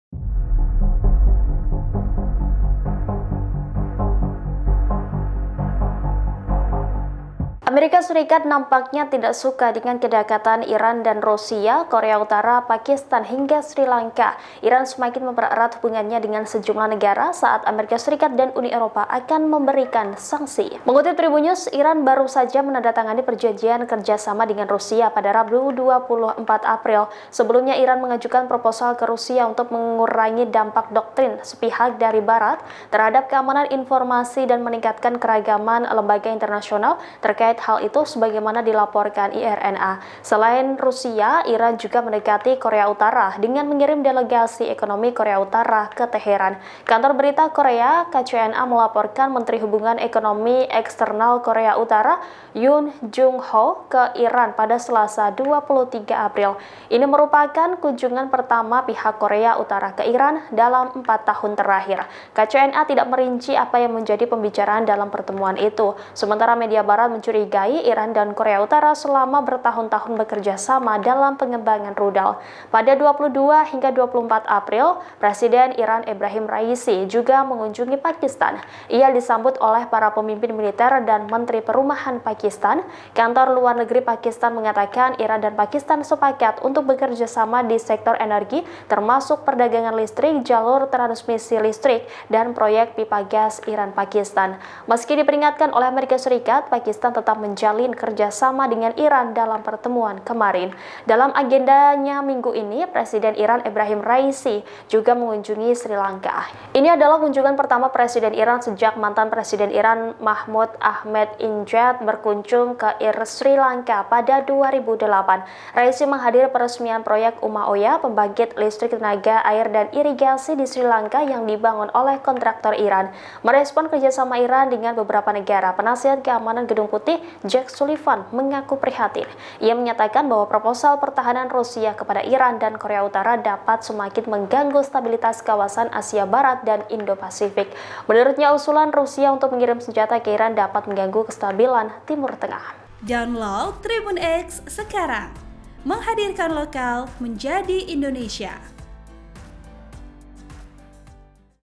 AS 'Kebakaran Jenggot' saat Tahu Iran Dekati Rusia, Korea Utara, Pakistan, hingga Sri Lanka
TRIBUN-VIDEO.COM - Amerika Serikat nampaknya tidak suka dengan kedekatan Iran dengan Rusia, Korut Pakistan hingga Sri Lanka.

Iran semakin mempererat hubungannya dengan sejumlah negara saat AS dan Uni Eropa akan memberi sanksi.